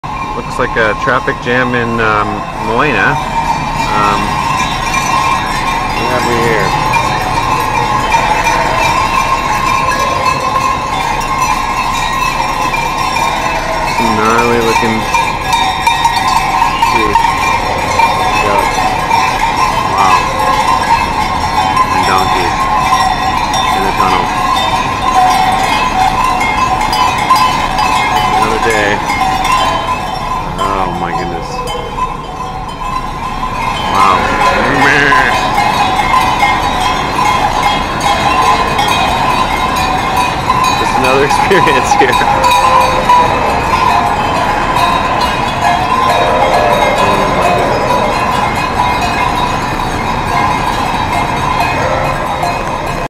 Beware the sheep! Traffic Jam in a Dolomiti Tunnel.
While guiding a cycling trip in the Italian Dolomites, I ran into a giant flock of sheep, goats, donkeys, etc. moving from Moena through a mountain tunnel, which stopped traffic for a good while. I saw them resting and eating mountain grasses the next day on Passo Fedaia.